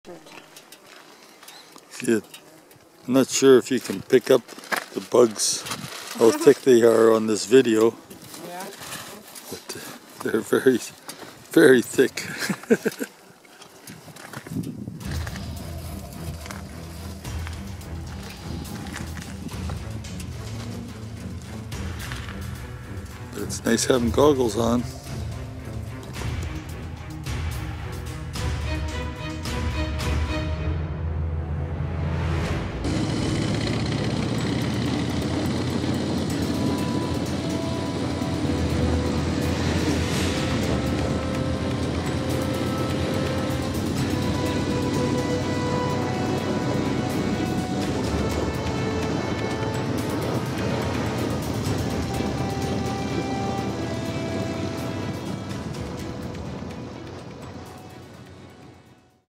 Harleys Crossing the Mackenzie River to Yellowknife
The wife and I have logged many miles on our Harleys; traveling to Sturgis, British Columbia, the Yukon and Northwest territories, to name a few, but the regions we've enjoyed the most were northern.

This video tracks us going over the enormous "Deh Cho" bridge, soon after it was built to cross the Mackenzie River in the Northwest Territories on our way to Yellowknife.  Prior to the bridge, the only way across was using a ferry (shown beached near the end of this video).

By the way, did you enjoy the music in this video?